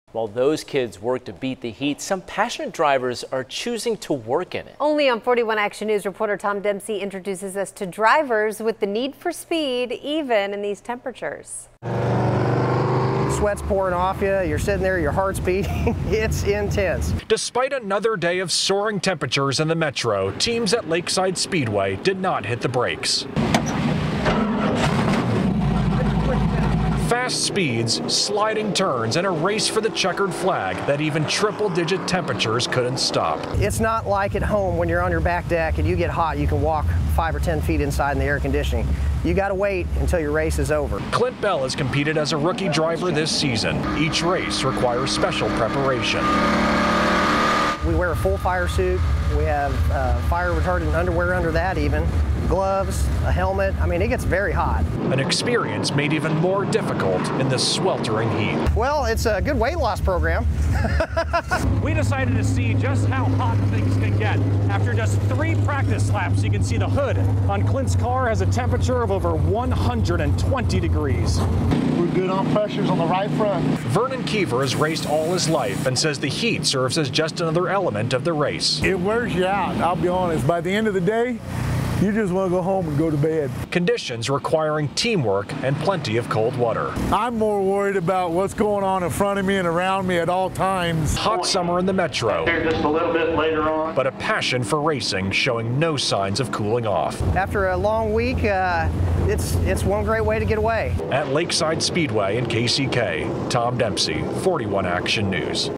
Hot weather in metro doesn't stop Lakeside Speedway drivers
Triple-digit temperatures in the metro couldn’t keep teams away from the racetrack at Lakeside Speedway on Friday.